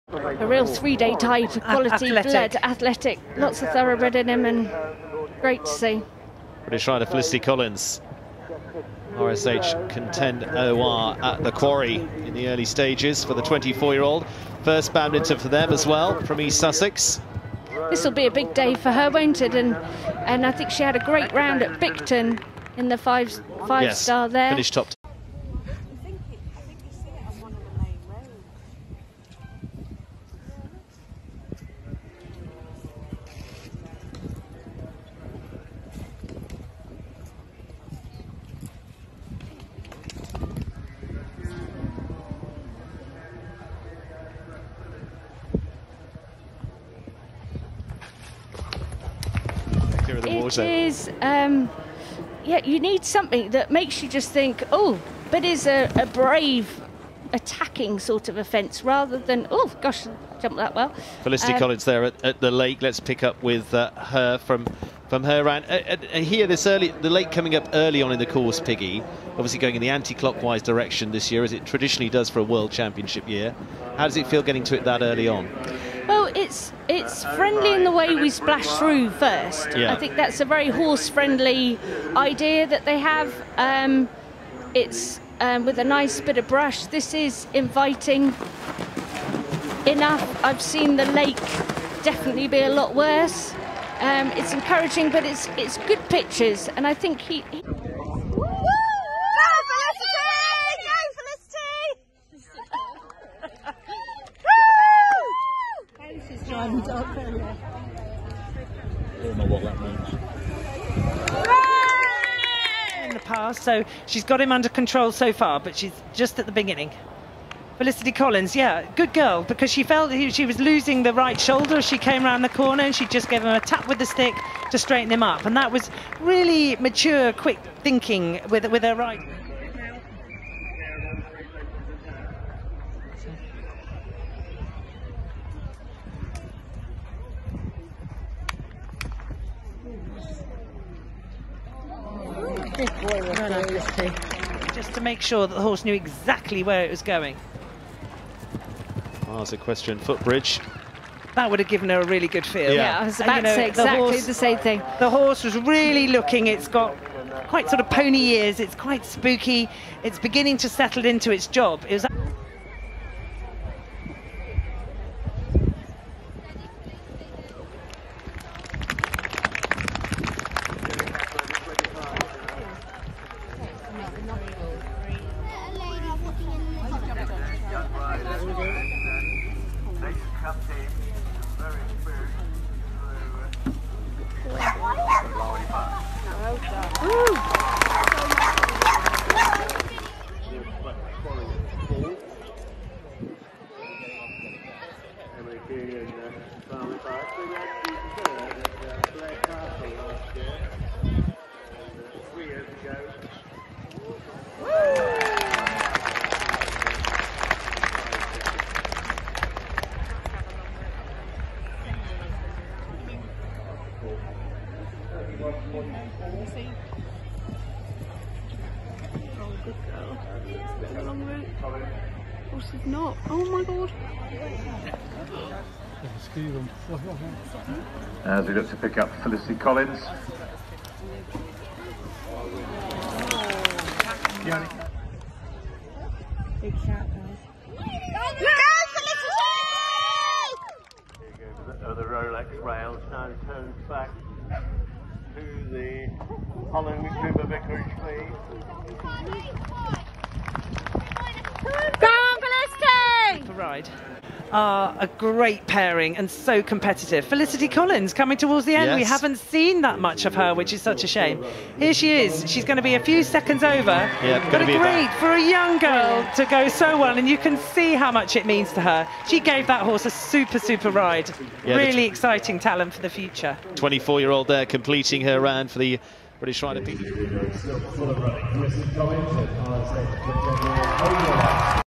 Badminton Cross Country 2022 - Felicity collins & RSH Contend'Or
Jumping clear around our Badminton debut!